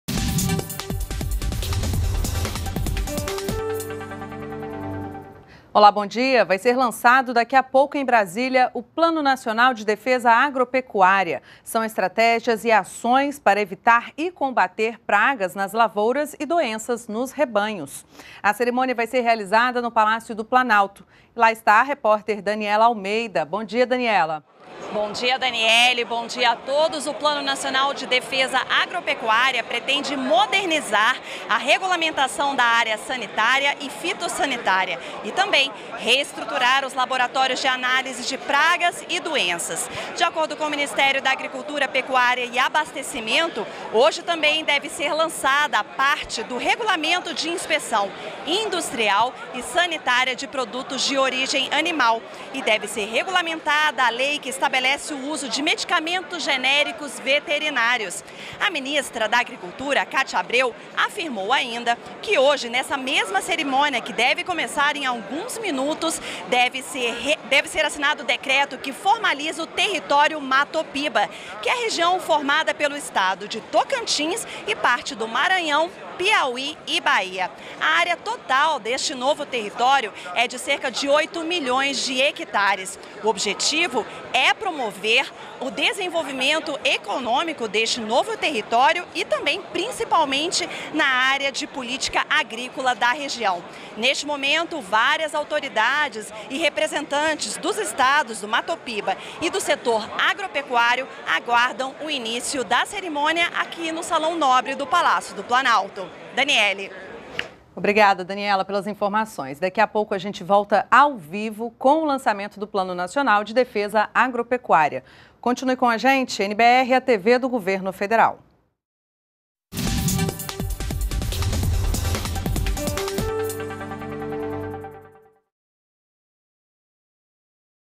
Governo lança Plano Nacional de Defesa Agropecuária
ATIVIDADES DA PRESIDENTA - 06.05.15: A presidenta Dilma Rousseff lança, nesta quarta-feira (6), o Plano Nacional de Defesa Agropecuária. São estratégias e ações para evitar e combater pragas nas lavouras e doenças nos rebanhos. O plano pretende modernizar a regulamentação da área sanitária e fitossanitária, e neste lançamento apresenta o conceito "Técnicas mais Avançadas, Alimentos mais Seguros, Consumidores mais Protegidos."